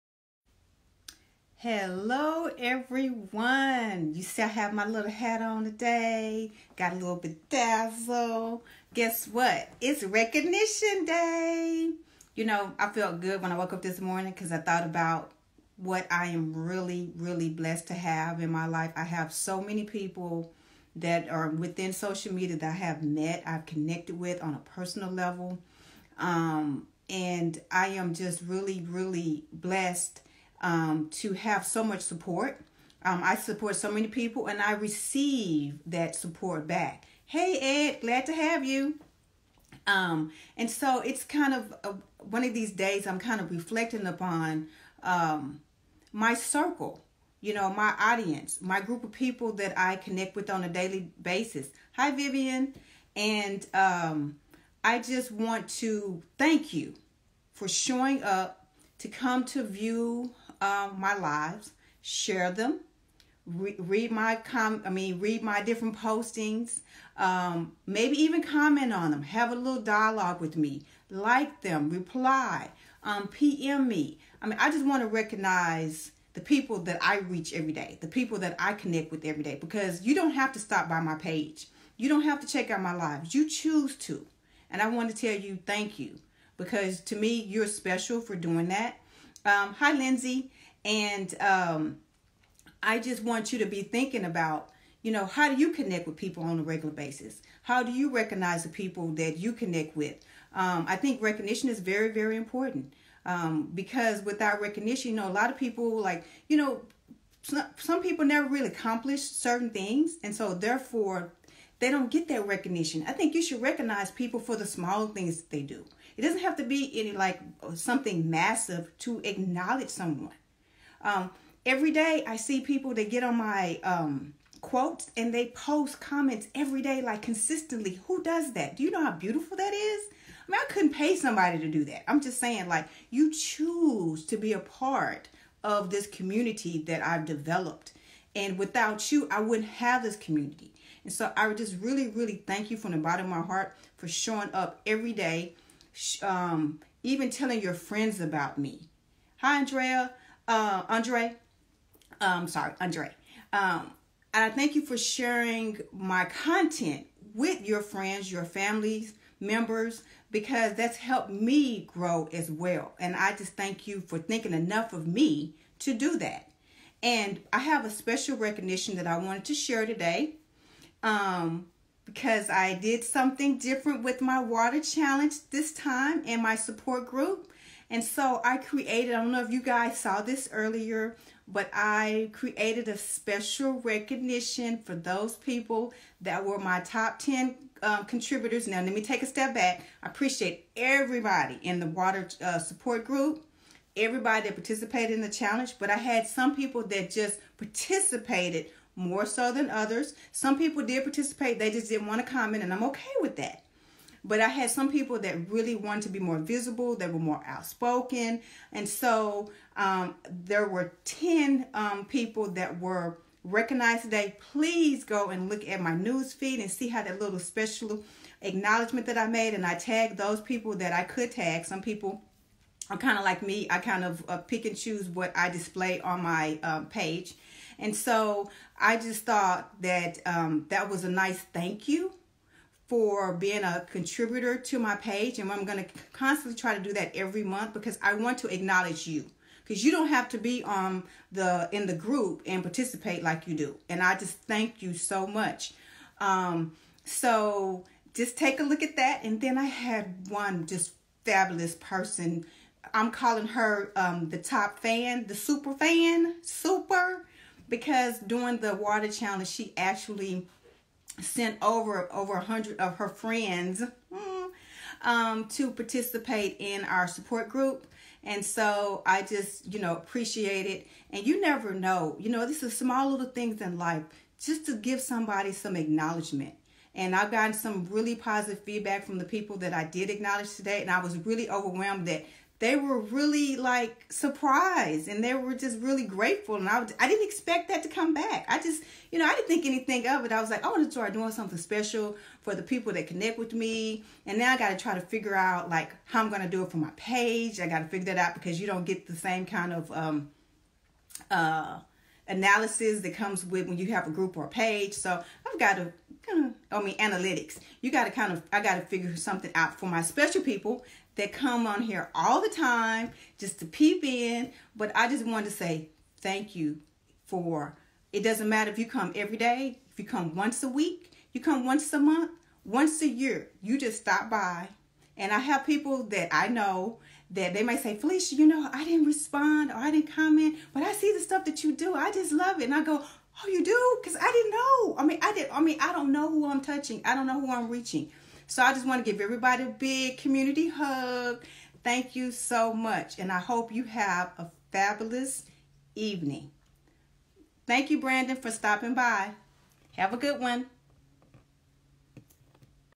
It's Recognition Day!
There are those people who consistently show support for you no matter what, be it online or offline. Give them recognition, you owe it to them. Acknowledge their presence, their effort, for taking the time to comment or share or like what you are doing on your social media.

The same thing in real life situation, don't hesitate to recognize these people who show support in all your endeavours. Thank them for helping you to grow as a better person.

I am a lifecoach, educator, social media enthusiasts, and a brand ambassador,.